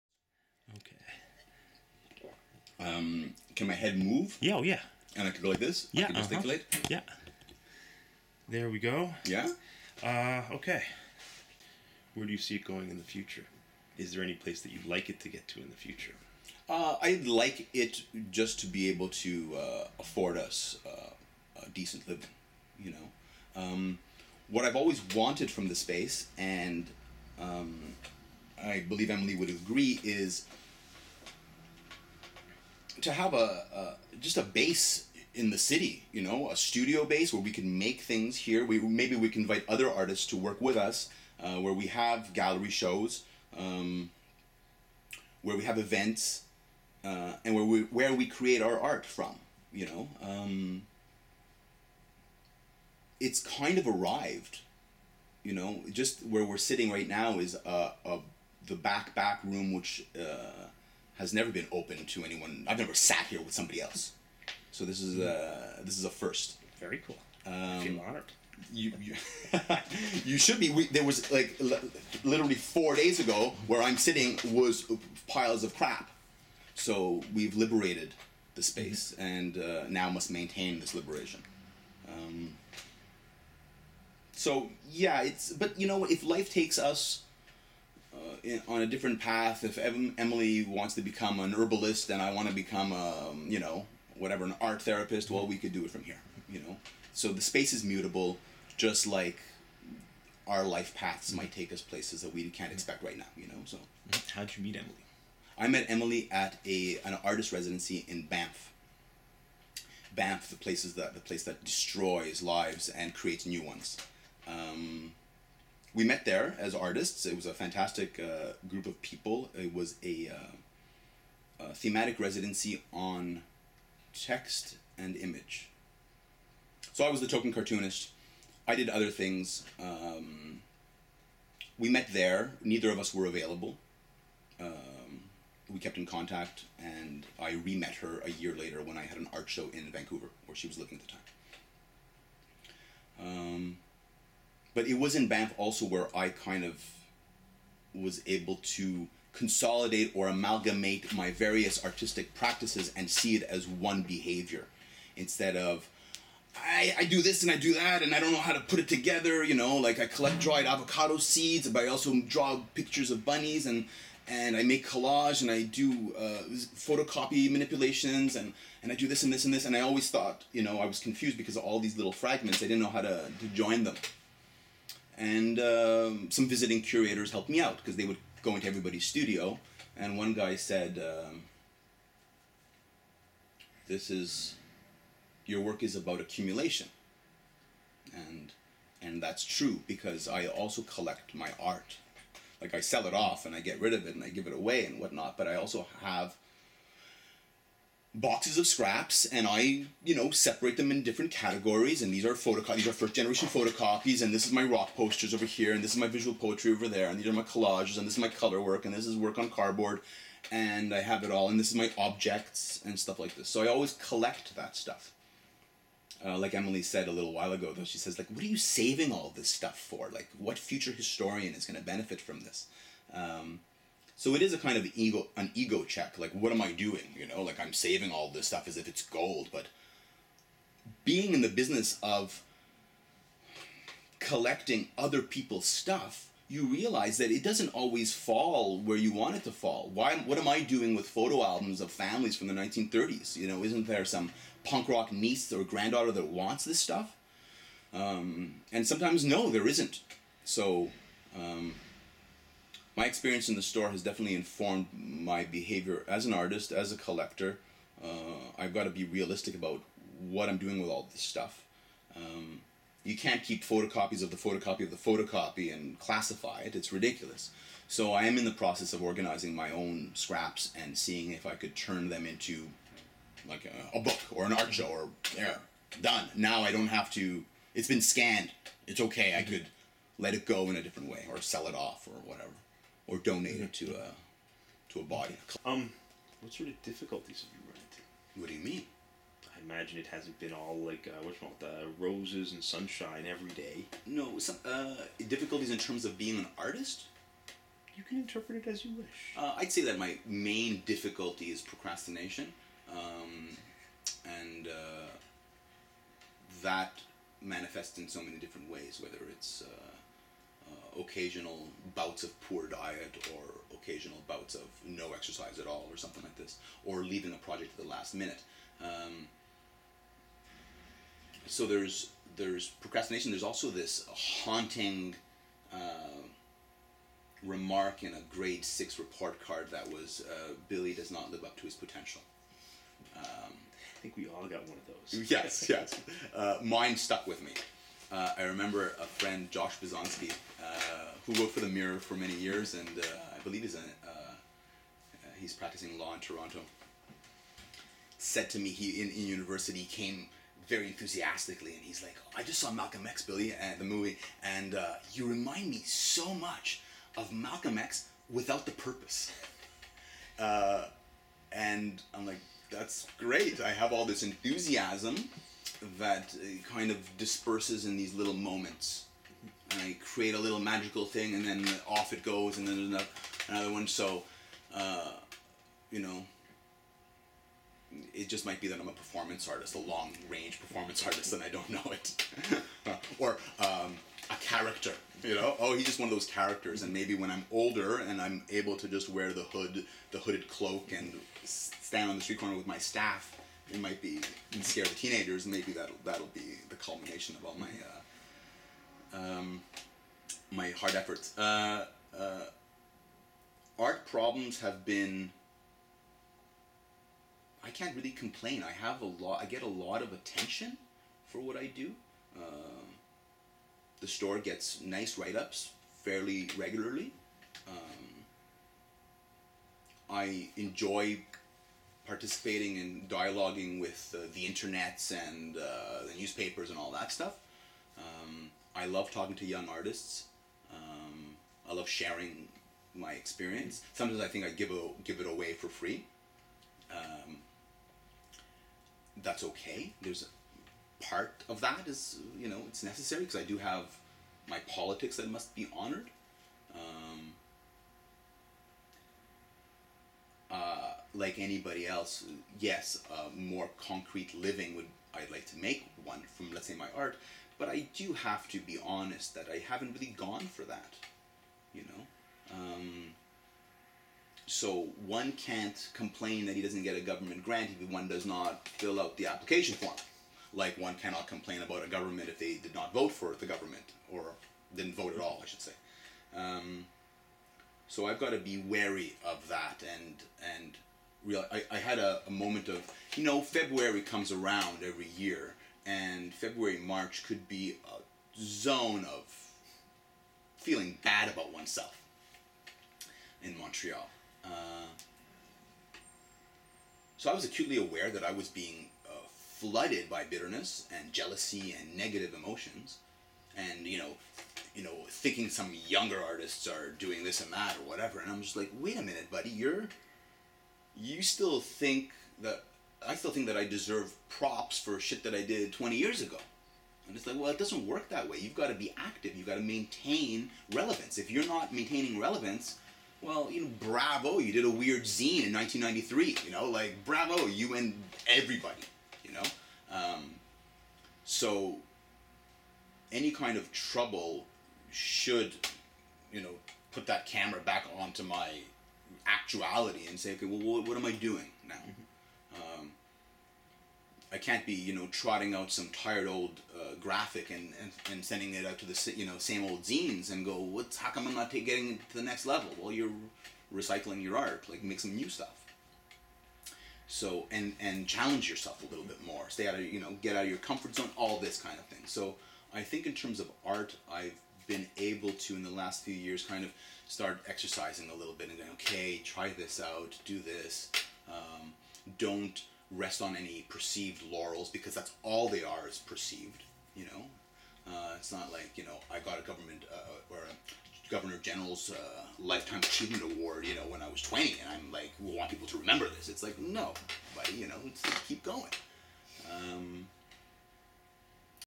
The Billy Mavreas Interview (Part Three)
Chris 'Zeke' Hand talks to Billy Mavreas, Montreal Artist about his career.